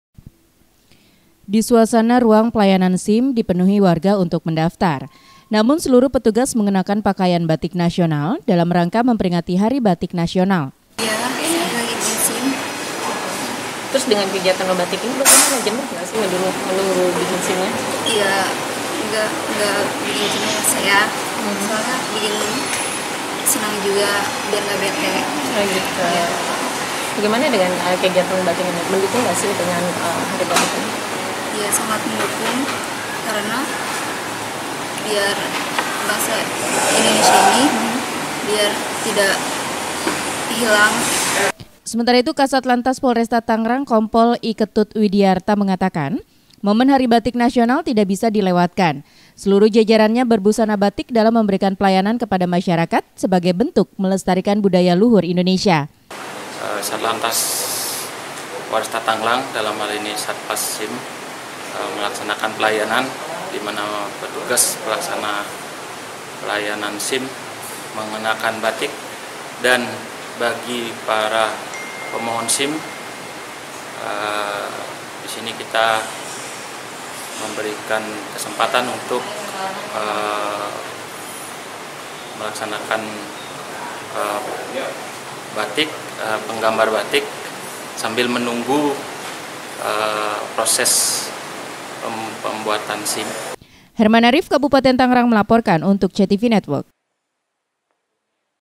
Satlantas Polresta Tangerang Ajak Masyarakat Membatik
MOMEN HARI BATIK NASIONAL, SATUAN LALU LINTAS POLRESTA TANGERANG SELURUH PETUGASNYA  BERBUSANA BATIK, DALAM MEMBERIKAN PELAYANAN KEPADA MASYARAKAT, SEKALIGUS MEMBATIK.